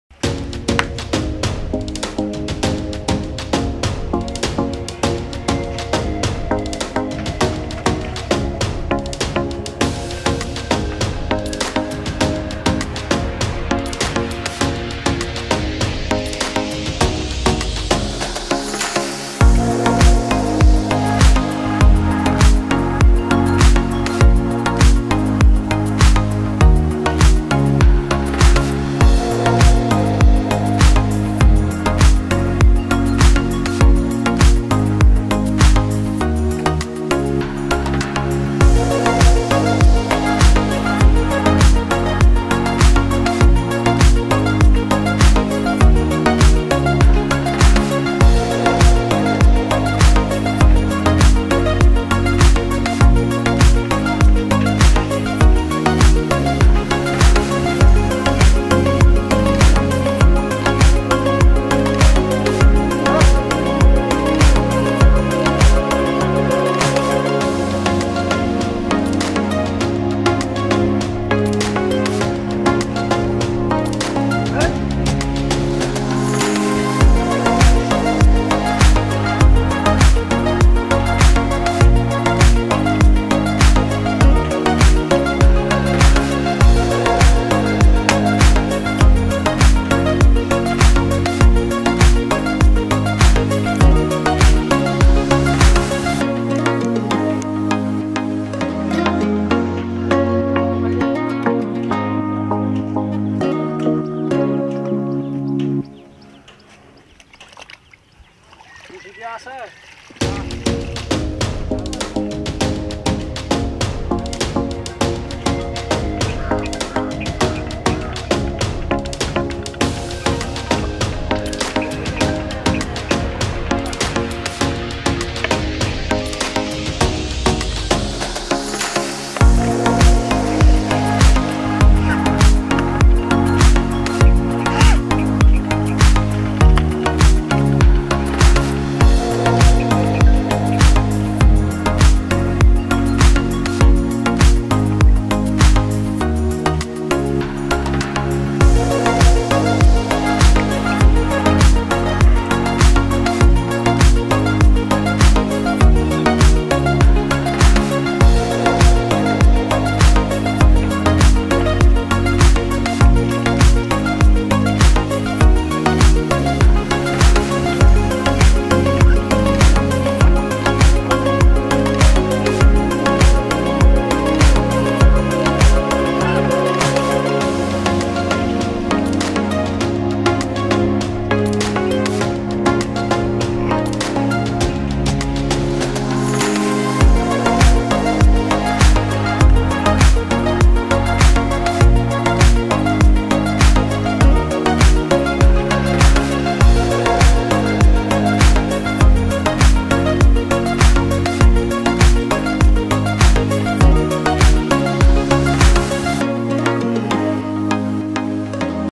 the process of gaining fat by fattening Bigest cows । Cow unloading at very village cattle market ।
Welcome to my The Arif Show  Channel...